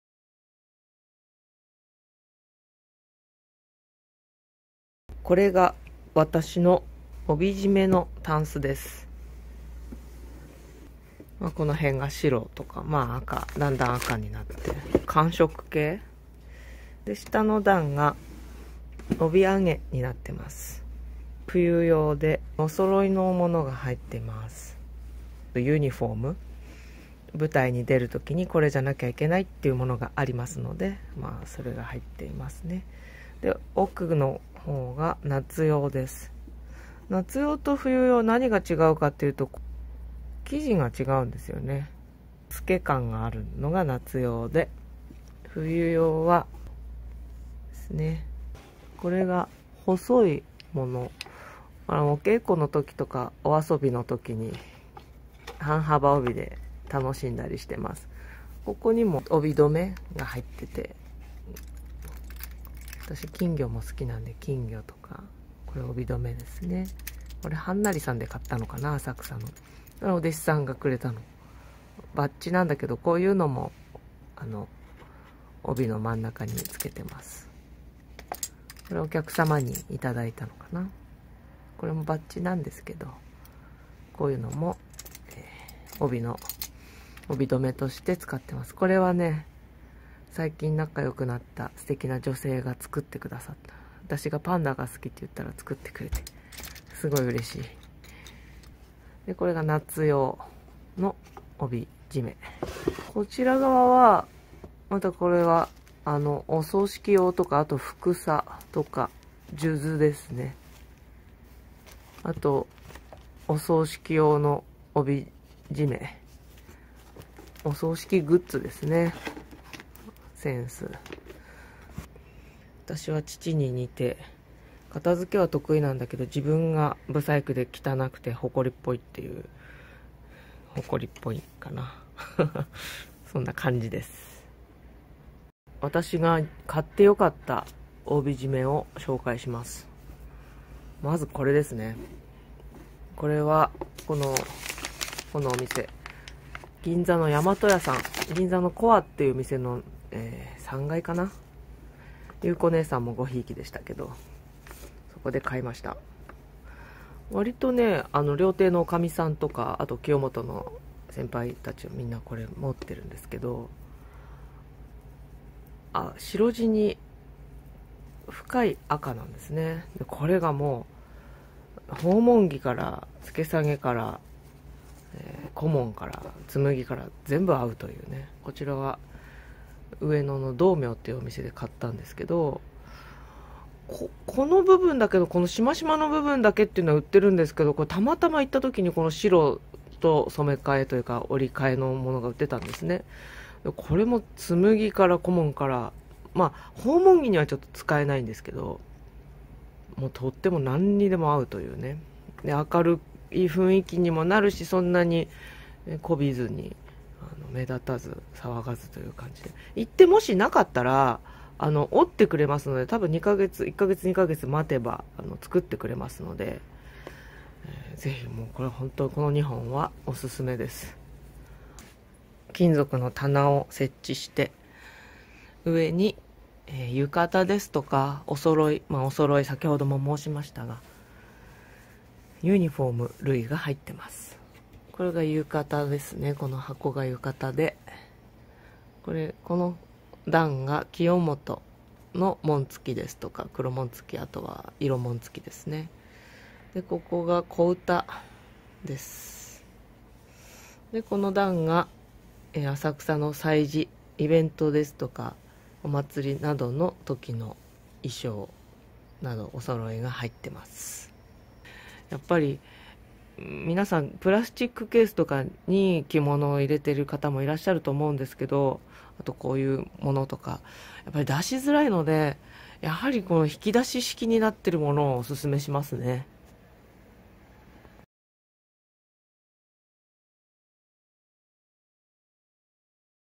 浅草紫沙 和服 買ってよかった帯締めランキング！帯締め帯揚げ揃い物の収納 How to store kimono accessories
帯締め、帯揚げの仕舞い方、買ってよかった帯締め、かわいい帯どめの紹介、揃い物の仕舞い方、たんすの良しあしなど

2021/2/20
春日とよ菊美　浅草紫沙　弾き歌い